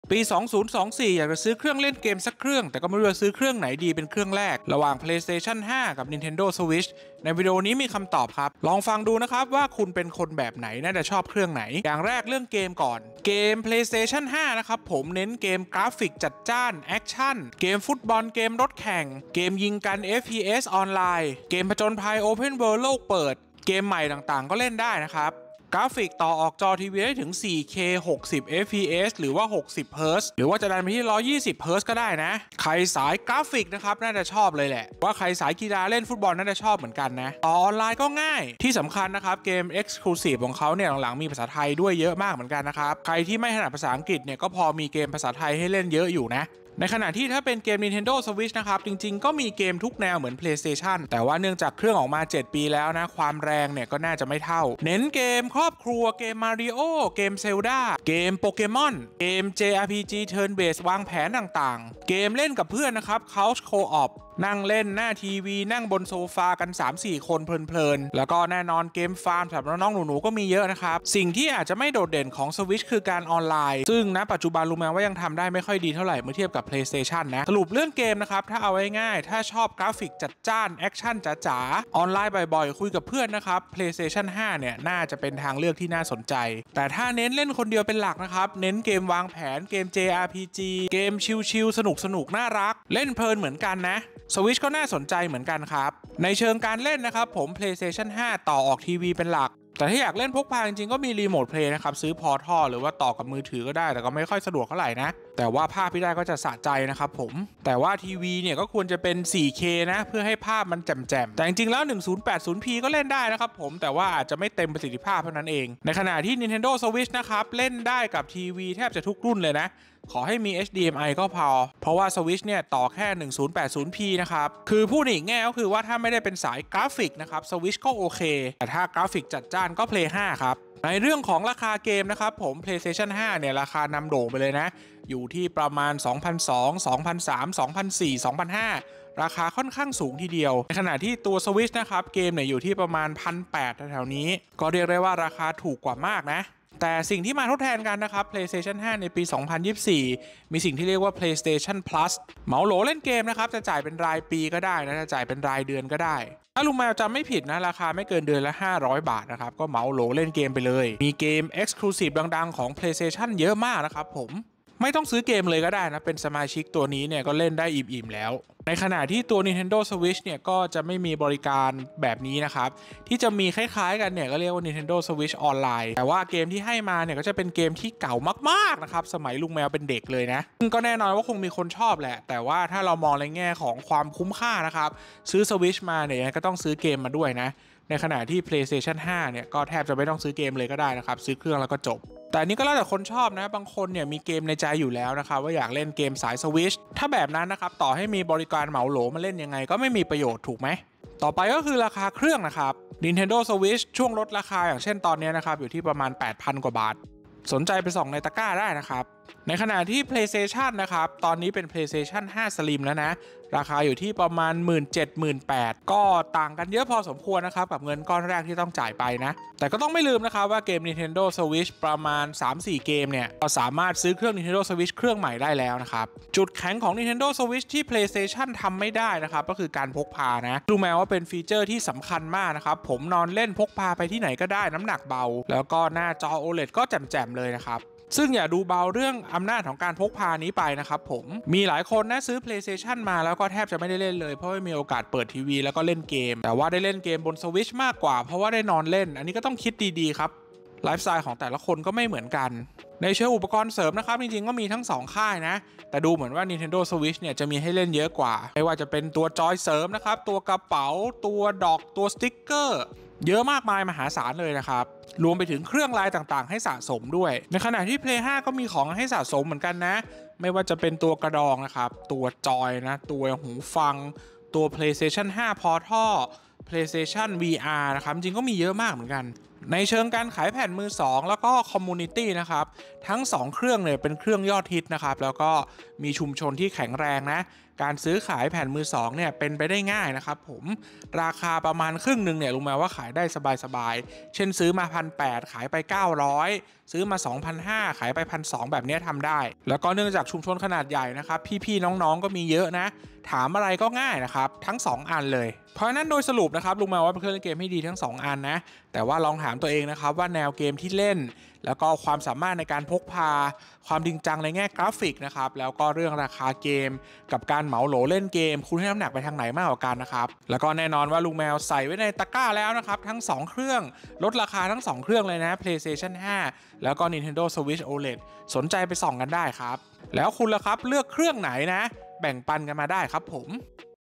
ซื้อ PS5 Slim หรือ Nintendo Switch ดี ในปี 2024 สำหรับคนซื้อเครื่องแรก
ใครกำลังลังเลว่าจะซื้อเครื่องไหน ต่างกันยังไง วันนี้ลุงแมวมาเปรียบเทียบแบ่งปันกันครับ PS5 กับ Switch ในแง่เกม การเล่น ราคา อุปกรณ์เสริม ชุมชน